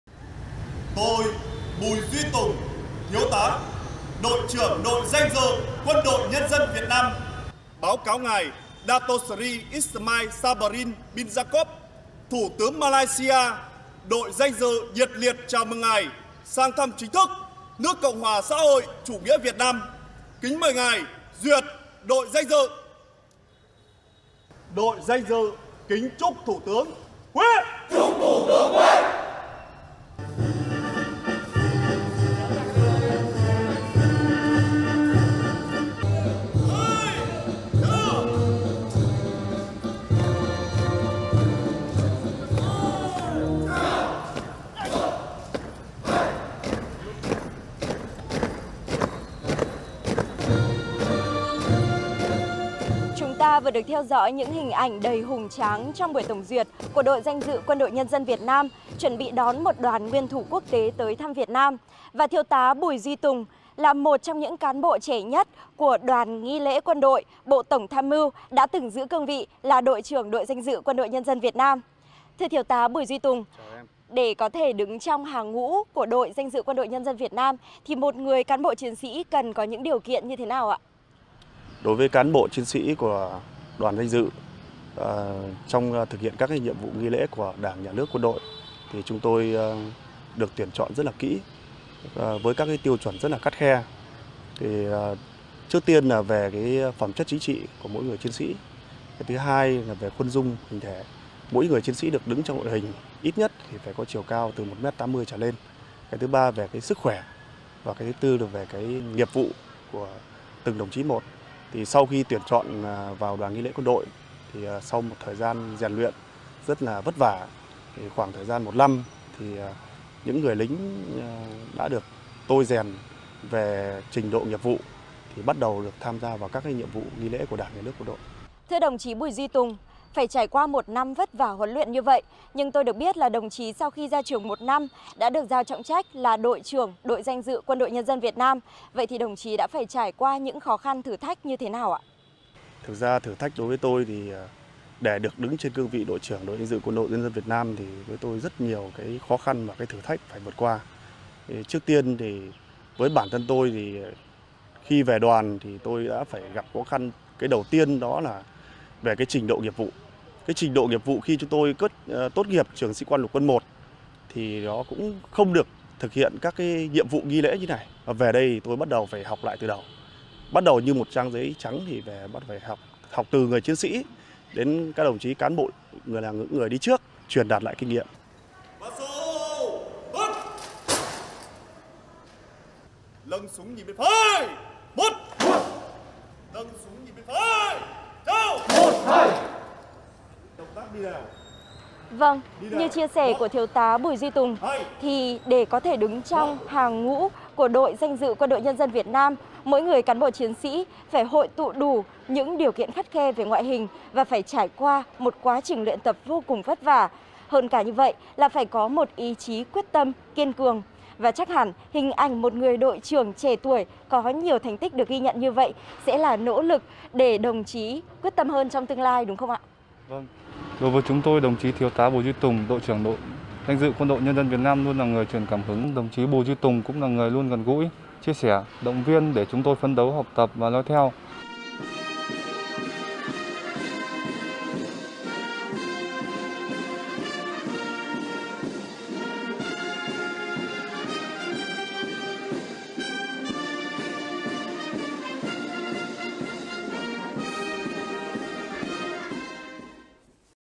Đội trưởng Đội Danh dự Quân Đội Nhân Dân Việt Nam
Để được chọn là Đội trưởng Đội Danh dự Quân đội nhân dân Việt Nam là một vinh dự tự hào nhưng không kém phần vất vả rèn luyện...